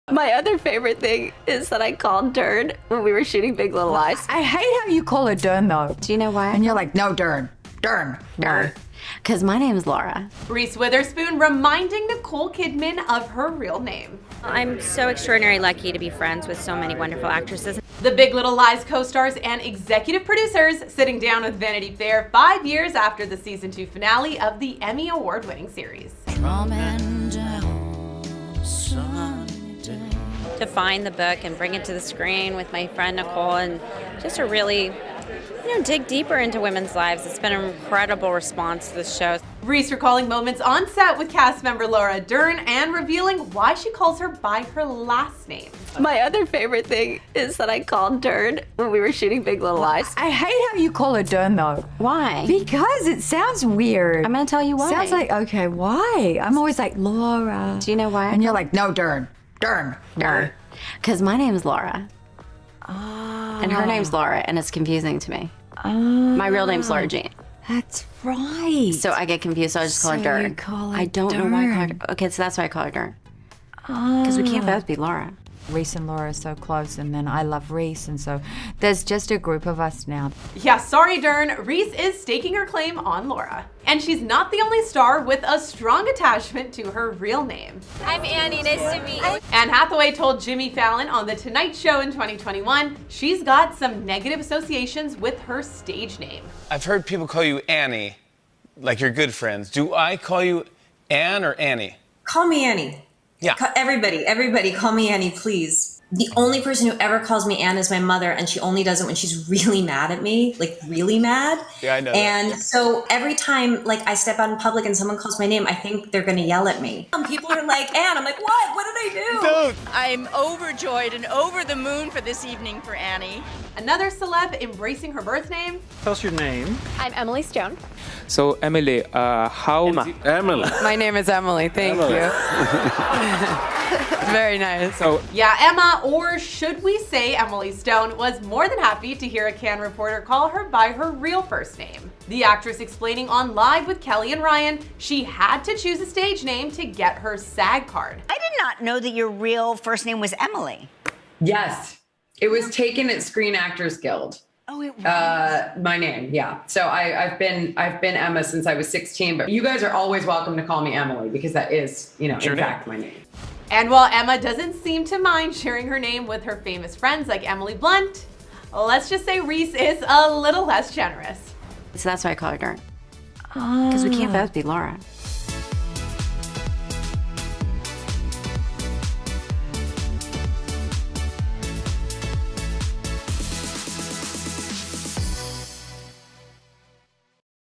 Reese Witherspoon REACTS to Nicole Kidman FORGETTING Her Real Name!
Reese Witherspoon reacts to Nicole Kidman forgetting her real first name as 'Vanity Fair' reunites the friends and co-stars five years after the season 2 finale of 'Big Little Lies.' Reese explains why she refers to Laura Dern as simply 'Dern,' saying it���s because the two share the same first name. Reese isn't the only star with an attachment to her real name - Anne Hathaway and Emma Stone have opened up about preferring to be called something other than their stage names.